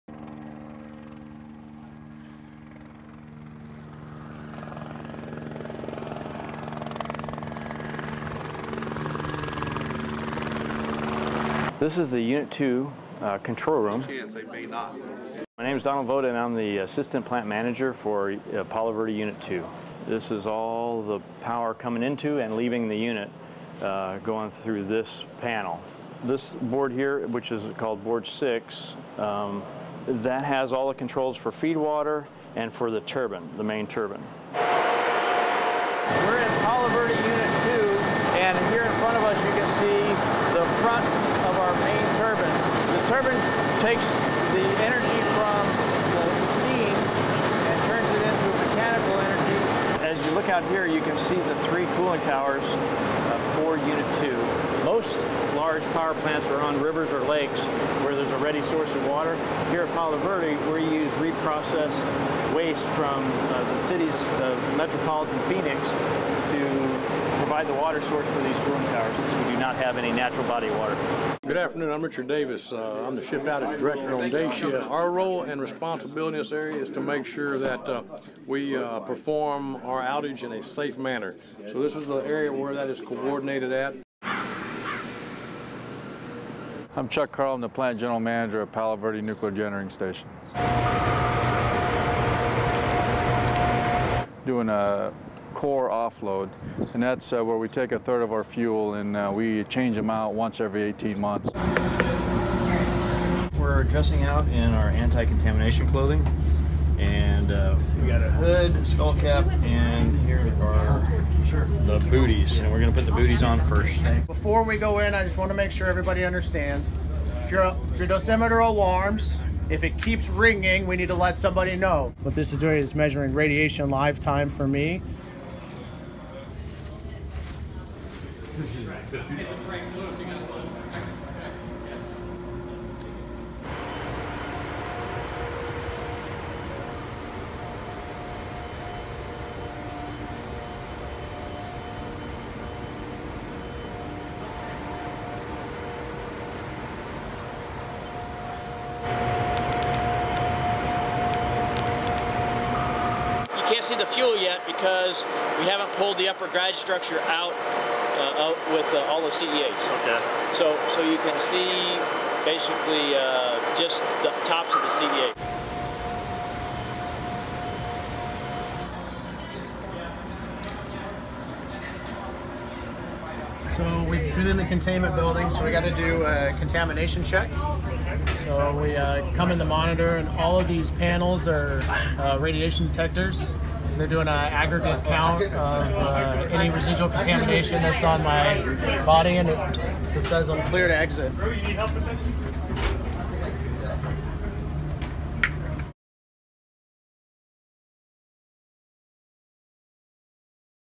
Inside a reactor at the Palo Verde Nuclear plant
It's the largest power plant of any kind in the United States, the Palo Verde Nuclear Generating Station is located 55 miles west of downtown Phoenix. It's operated by APS and employees over 2,000 people.